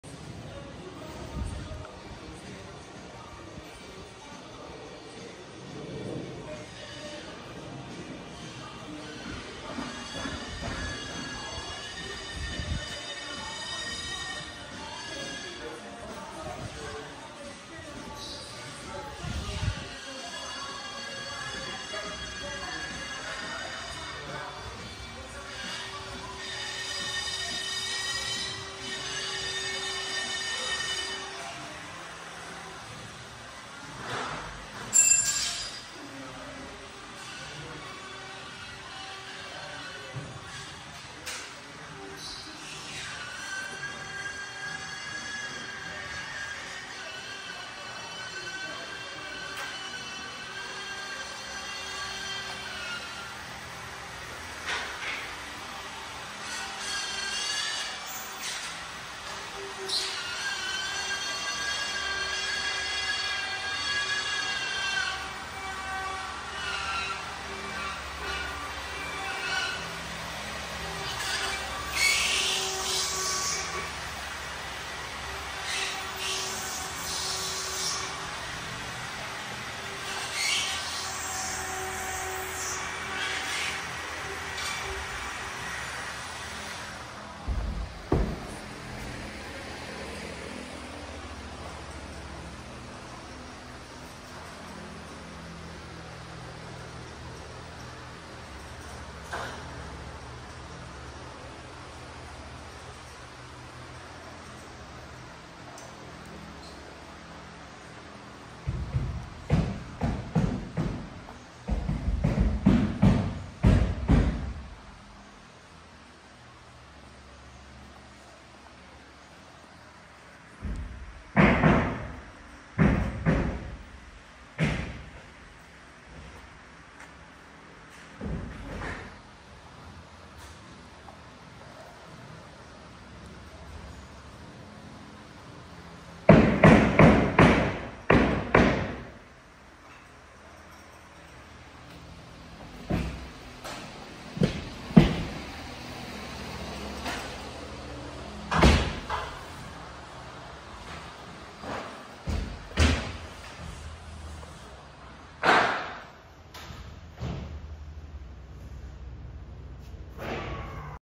Paintaholics - Painting Office Space
Paintaholics - Office Space 1st day
Priming walls at this office space project, interior painting. e will be doing accent walls and doors
Please see the finished video of the transformation.